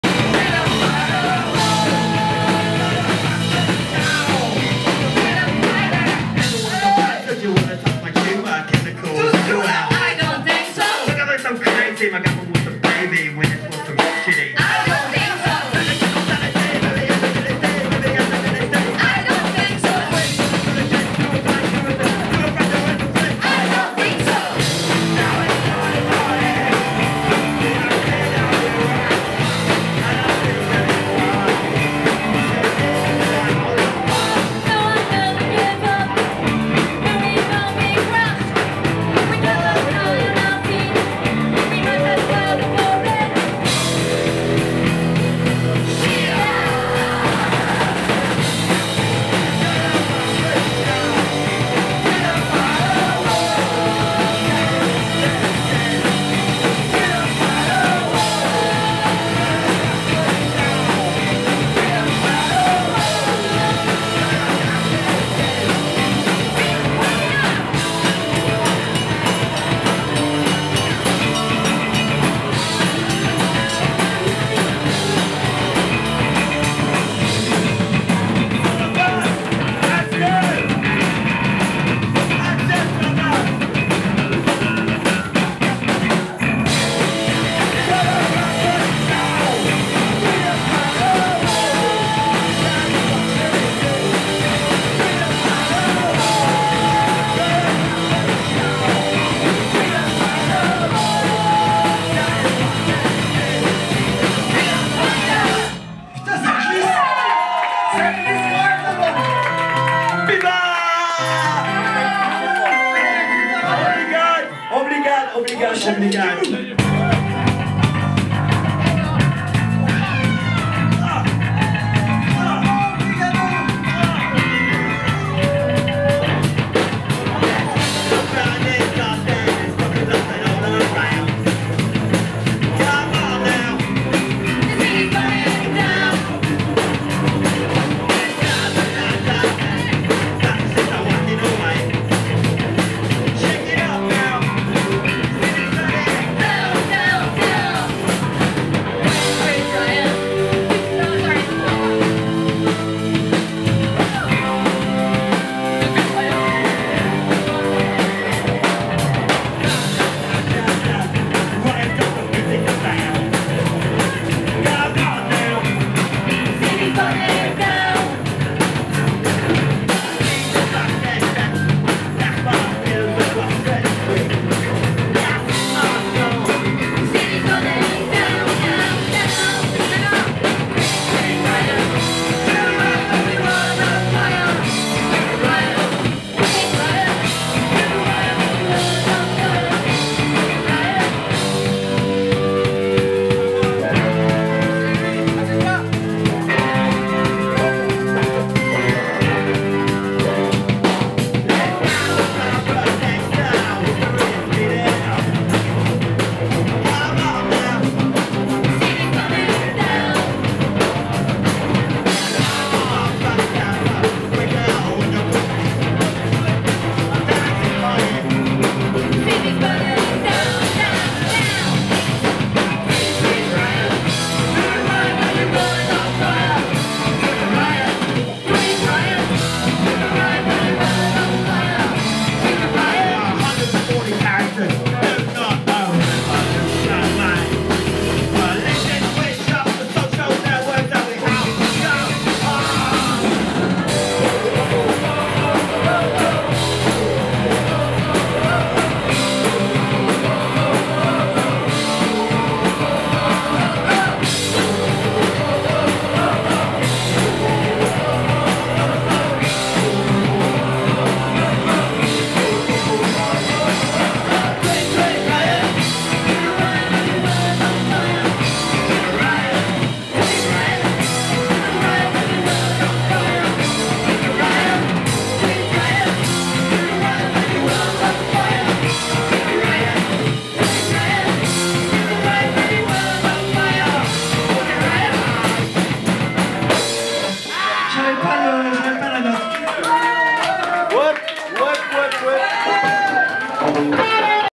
Louis Lingg & the Bombs
Le 31 octobre 2015 à la Comedia Michelet à Montreuil.